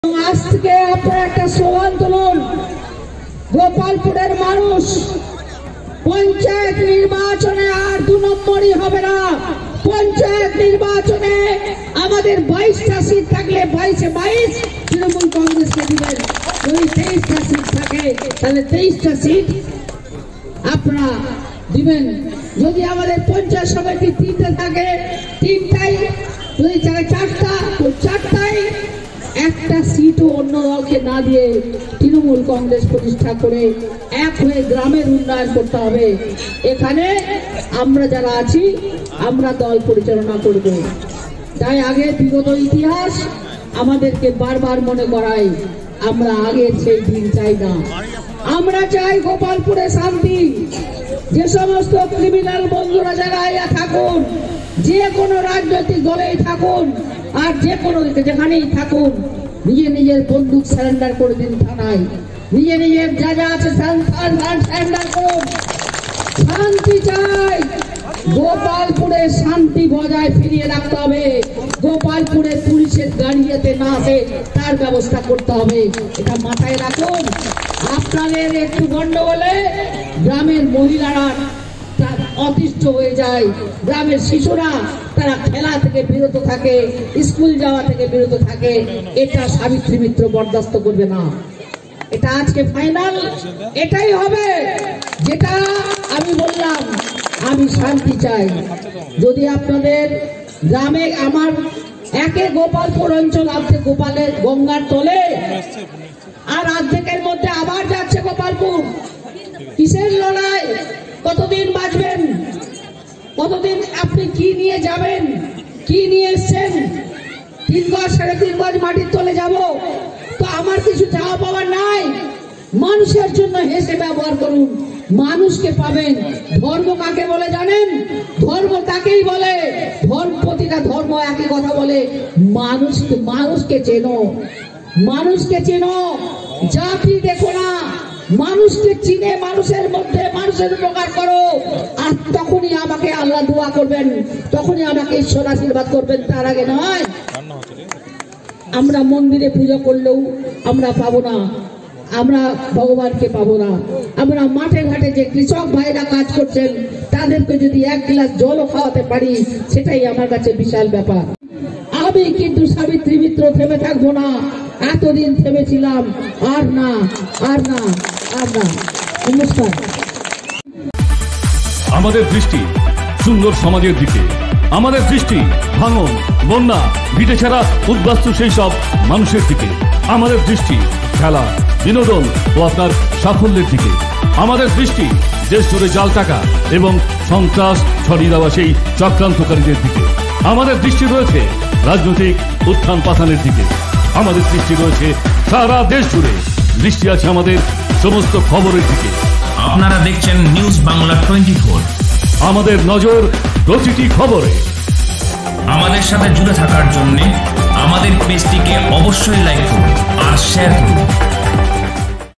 আমরা চাই গোপালপুরে শান্তি এমনি বার্তা দিলেন গোপালপুরের যোগদান মঞ্চ থেকে বিধায়িকা সাবিত্রী মিত্র।
আমরা চাই গোপালপুরে শান্তি এমনি বার্তা দিলেন গোপালপুরের যোগদান কর্মসূচির মঞ্চ থেকে মানিকচক বিধায়িকা সাবিত্রী মিত্র।